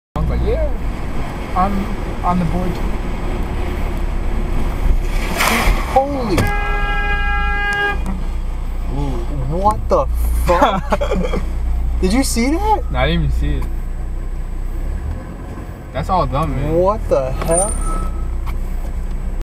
Bad Drivers of New Jersey
I guess the rainy conditions caused this driver to completely miss seeing me.